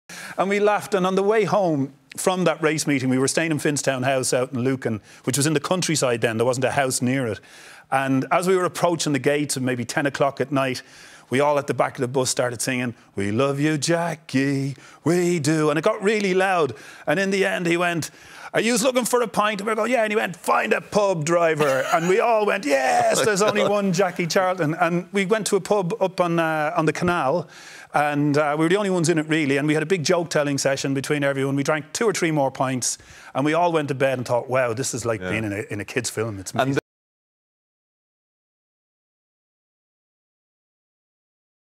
"Find a pub, driver!" - Jack Charlton story that summed up how much fun the Ireland players had
Coming home late on the bus, pulling up to their residence, the famous Ireland players from the 1990 and 1994 World Cups convinced Jack Charlton to keep the party going with a "we love you Jackie" chant.